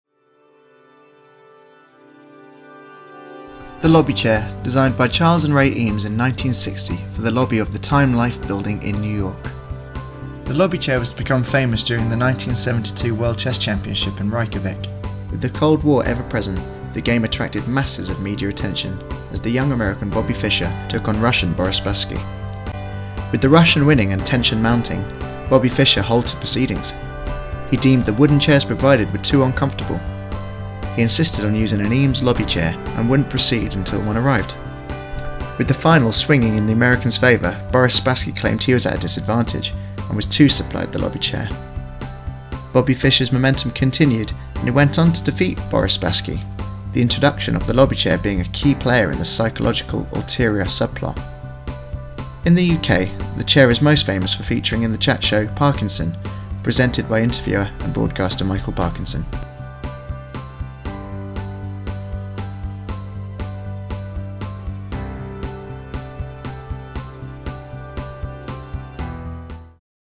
Eames Lobby Chair by Find Me The Original
Designed by Charles and Ray Eames in 1960 for the lobby of the Time Life building in New York

The Lobby Chair was to become famous during the 1972 World Chess Championship in Reykjavik.

With The Cold War ever-present the game attracted masses of media attention, as the young American Bobby Fischer took on Russian Boris Spassky.

With The Russian winning and tension mounting, Bobby Fischer halted proceedings.

He deemed the wooden chairs provided were too uncomfortable.

He insisted on using the Eames Lobby Chair and wouldn't proceed until one arrived.

With the final swinging in The American's favour. Boris Spassky claimed he was at a disadvantage and was too supplied the Lobby Chair.

Bobby Fischer's momentum continued and went on to defeat Boris Spassky.
The introduction of The Lobby chair being a key player in the psychological ulterior sub plot.

In the UK the chair is famous for featuring in the chat show 'Parkinson'. Presented by interviewer and broadcaster Michael Parkinson.